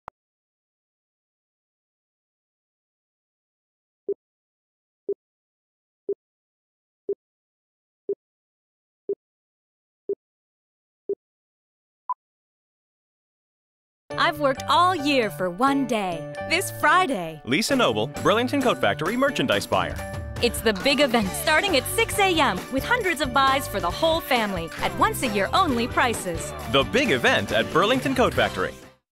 Burlington Coat Factory Commercial, Fall 2008 "black friday"
Featuring voice over by Serra Hirsch